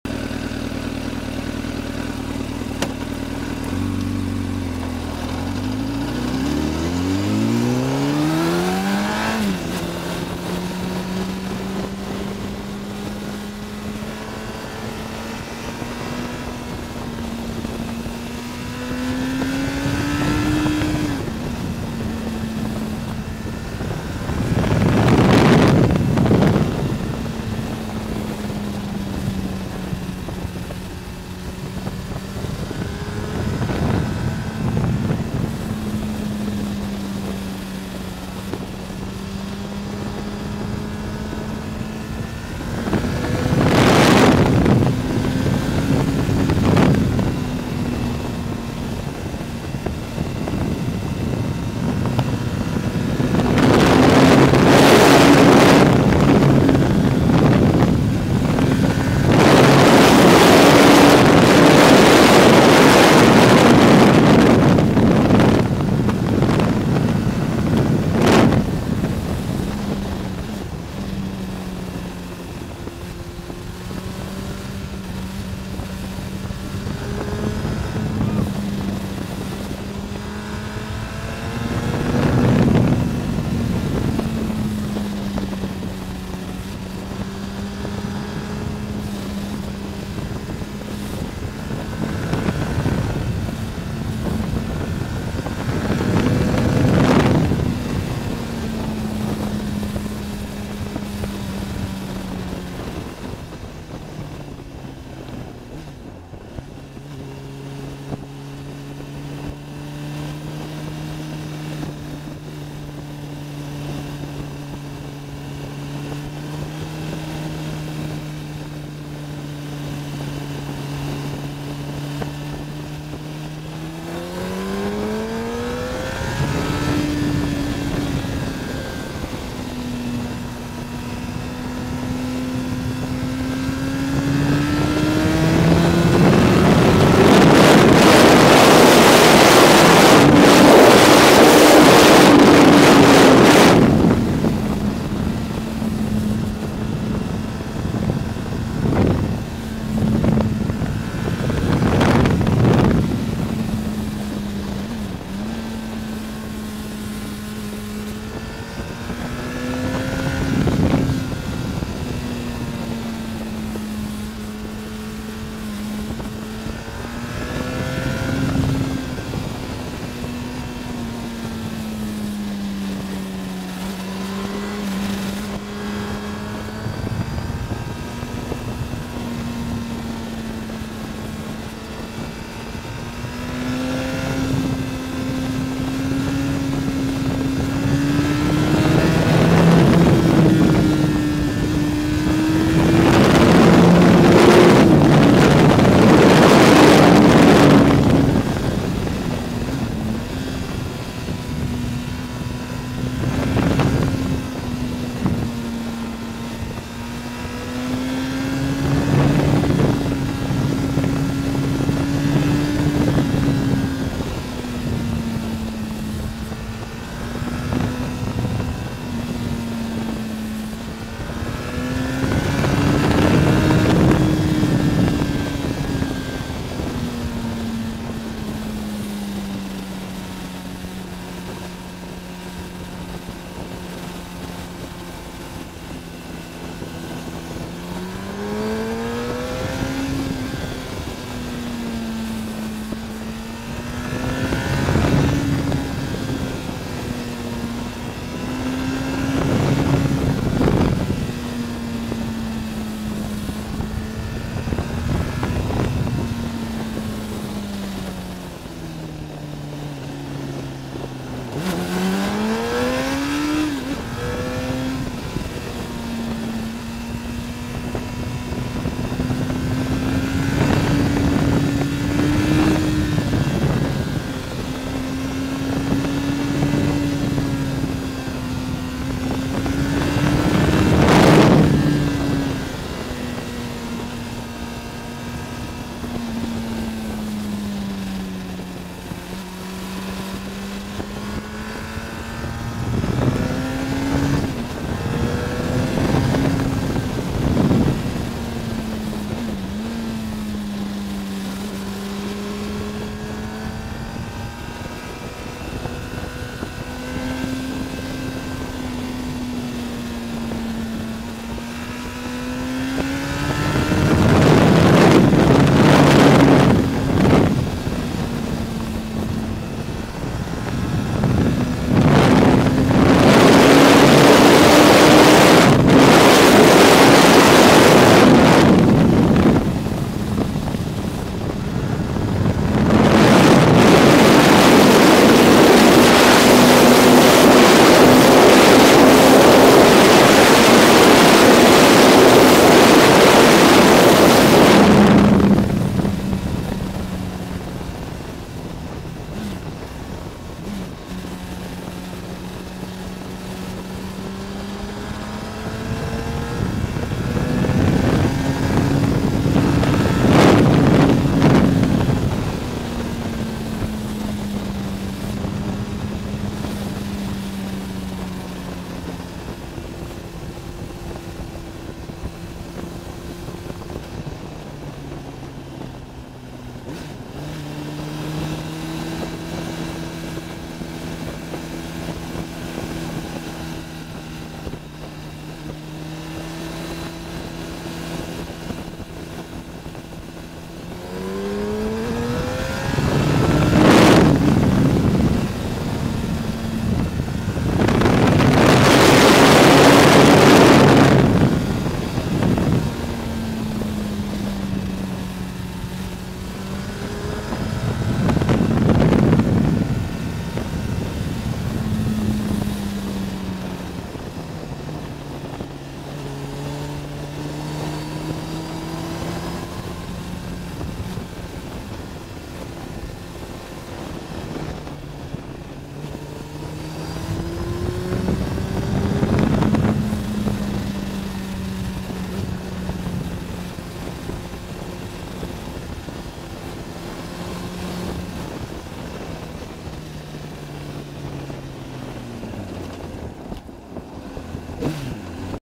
Honda CBR 1000RR Stare Drawsko - Połczyn Zdrój (163)
Droga wojewódzka nr 163 na odcinku Stare Drawsko - Połczyn Zdrój przecinająca Dolinę Pięciu Jezior zwaną  Szwajcarią Połczyńską. 122 zakręty.